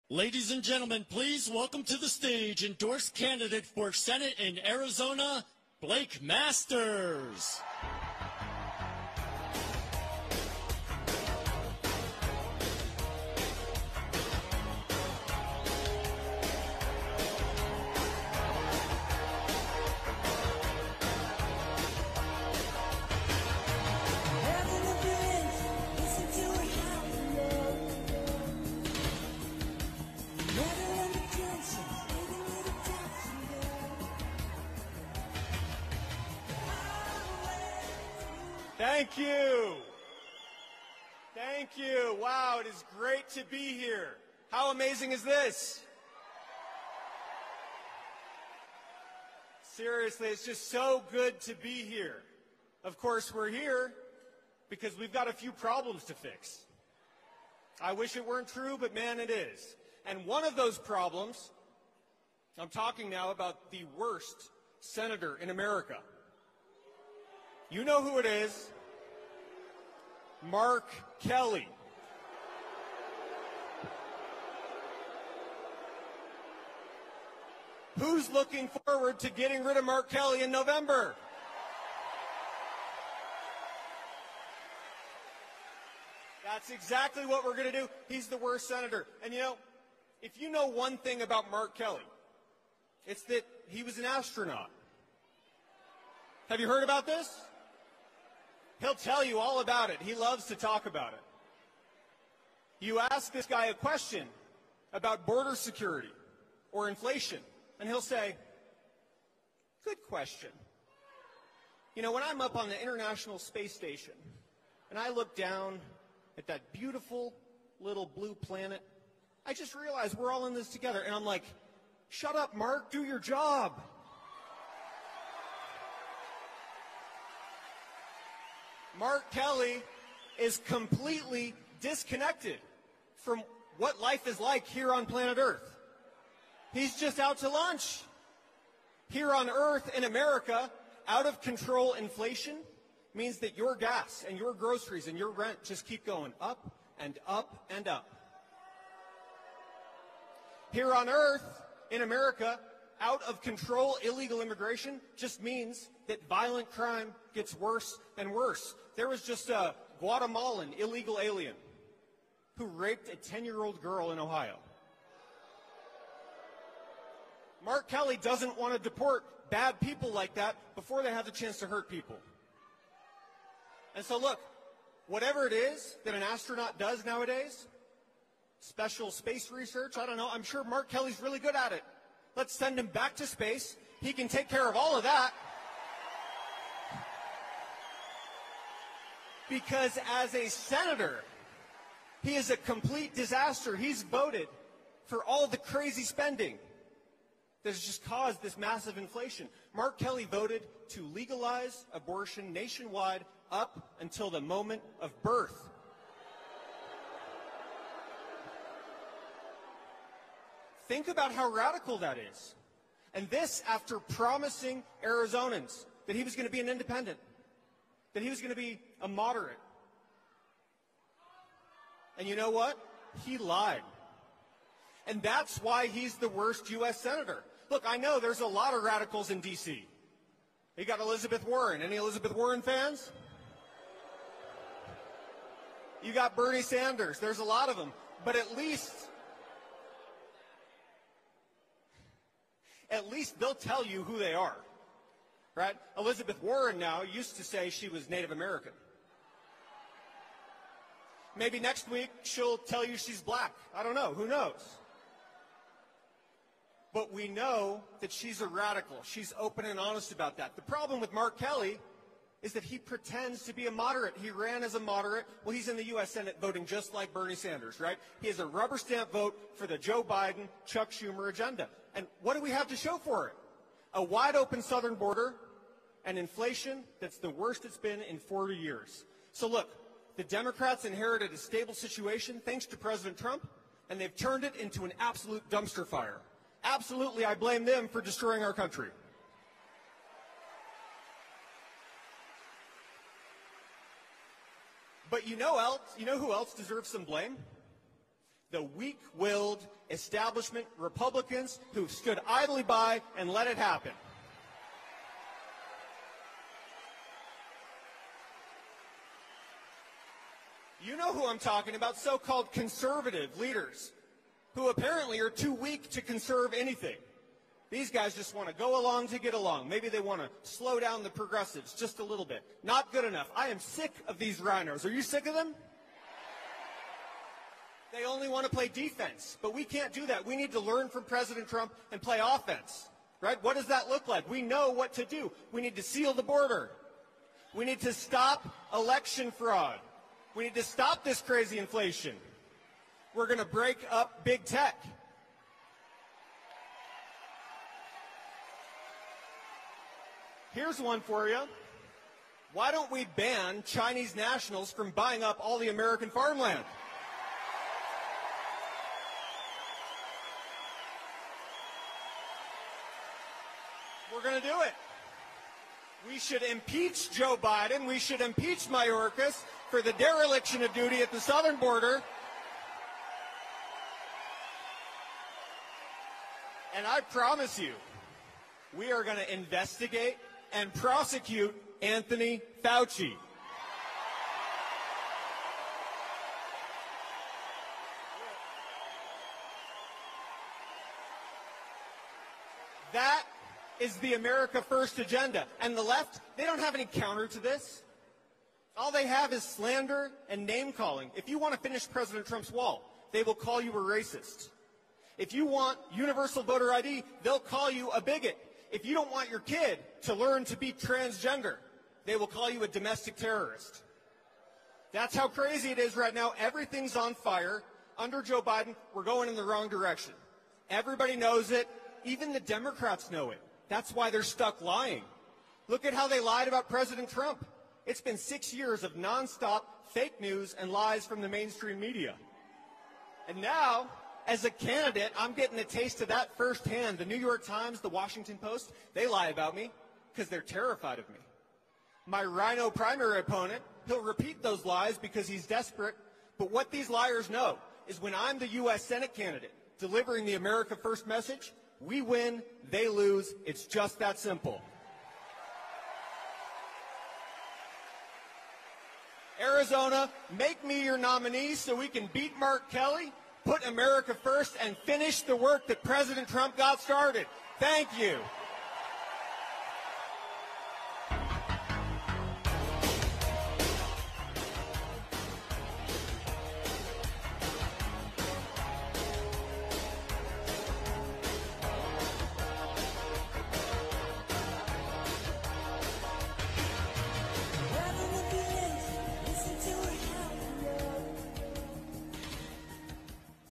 Blake Masters speaks at the Save America Rally
Blake spoke at President Trump's Save America Rally in Prescott, AZ.